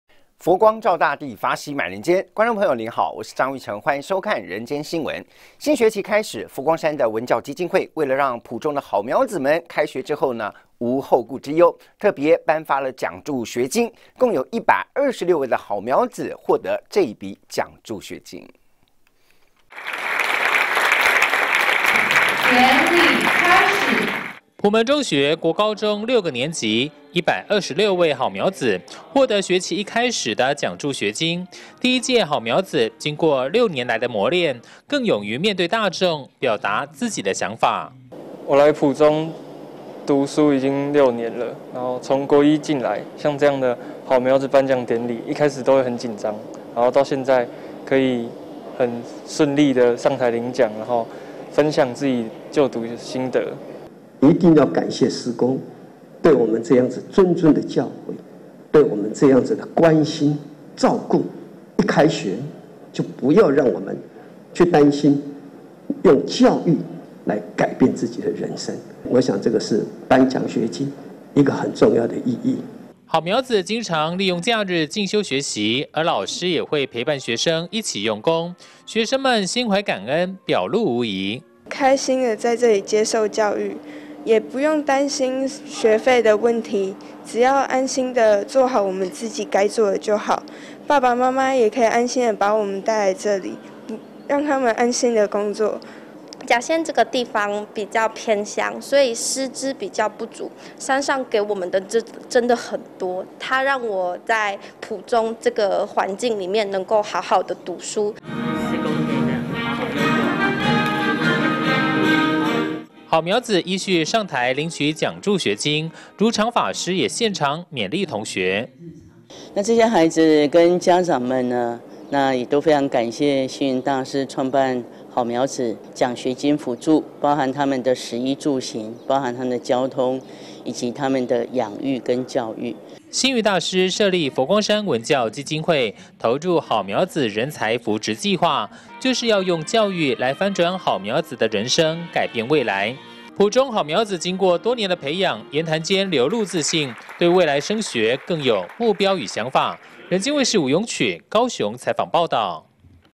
20220216 安心就學！普門中學頒發好苗子獎助學金
新學期開始，佛光山文教基金會為了讓普中好苗子開學無後顧之憂，特別頒發獎助學金，共有126位好苗子領取這筆獎助學金，師長們也到場為他們獻上祝福。